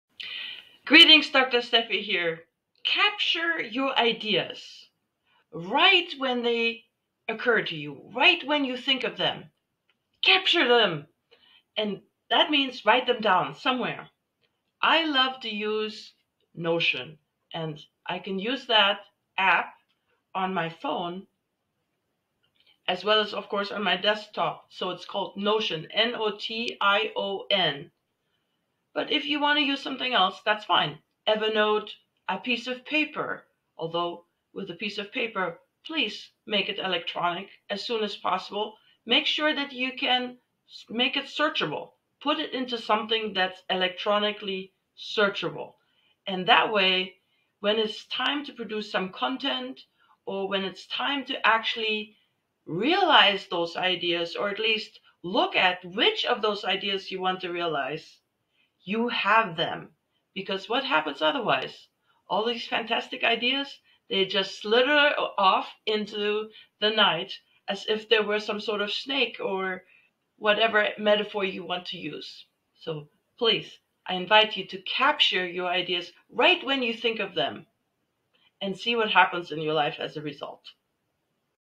Capture Your Ideas Right When You Think Of Them - The Brilliance Mine
Greetings, Dr. Stephie here. Today, my nugget is this: **Capture your ideas** right when they occur to you. Right when you think of them, capture them.

That means write them down somewhere. I love to use **Notion**. I can use the Notion app on my phone and, of course, on my desktop. It’s called [Notion]

If you want to use something else, that’s fine:

- Evernote
- A piece of paper – although with a piece of paper, please make it electronic as soon as possible.

**Make sure that you can make it searchable**. Put it into something that’s electronically searchable. That way, you have your ideas readily at your fingertips when it’s time to

- Produce content
- Look at which of those ideas you want to realize
- Realize the ideas you have chosen

Because otherwise, all these fantastic ideas you had, just slipped off into the night as if they were some sort of snake or whatever metaphor you want to use.

Don't forget to leverage YOUR brilliance.
Cheers
Dr. Stephie